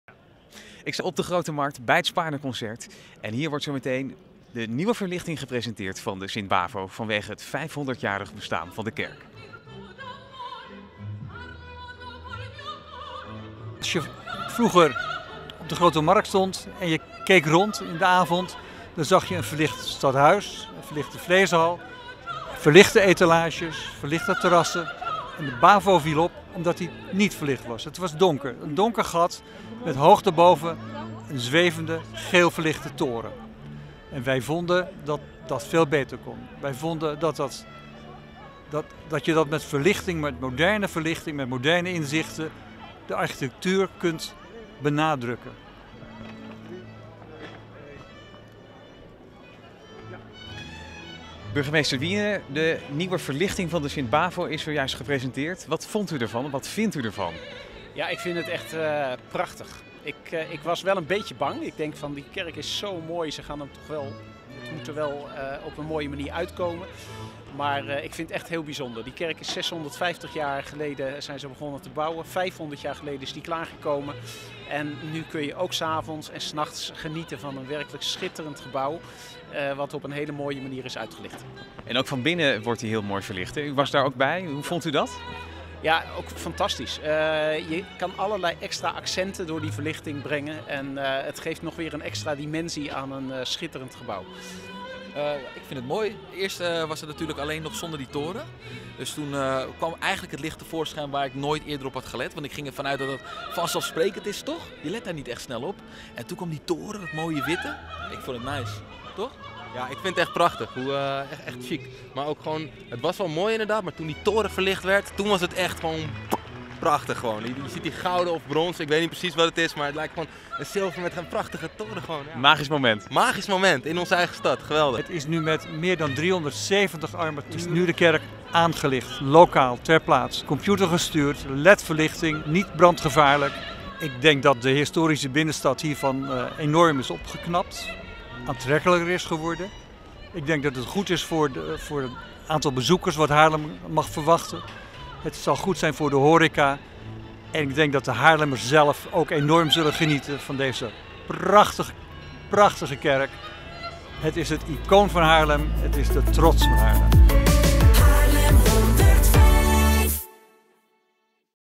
Presentatie nieuwe verlichting oude Bavo kerk
Tijdens een aangepaste versie van het Spaarneconcert wordt de nieuwe verlichting van de oude Bavo kerk gepresenteerd.